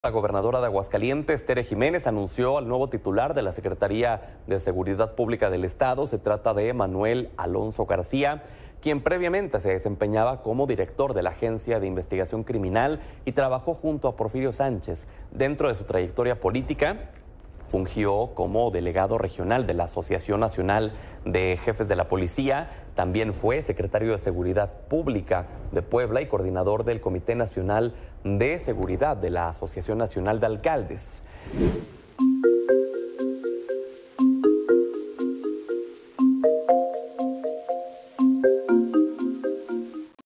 Nombran a nuevo secretario de Seguridad de Aguascalientes tras muerte de Porfirio Sánchez
La gobernadora de Aguascalientes, Tere Jiménez, anunció quién ocupará la titularidad de la Secretaría de Seguridad Pública del estado, luego de que Porfirio Javier Sánchez Mendoza falleció en un desplome aéreo.